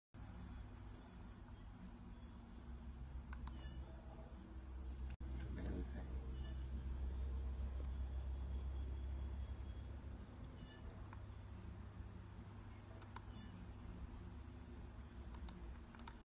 นั่งกลางถนนหลวง ดีไม่มีรถชน ตำรวจพาไปโรงพักแล้ว
ฝากเตือนผู้ใช้รถใช้ถนนเพิ่มความระมัดระวังในการเดินทางค่ะ

วันนี้คุณพ่อเดินทางจากฝางเข้าเมืองเชียงใหม่
ช่วงประมาณ11.30น.
เลยเชียงดาวฮิลล์มาเล็กน้อย พบมีคนนั่งอยู่กลางถนนตามคลิป
จึงส่งคลิปมาเพื่อให้ผู้ใช้รถใช้ถนนเพิ่มความระมัดระวังในการเดินทางค่ะ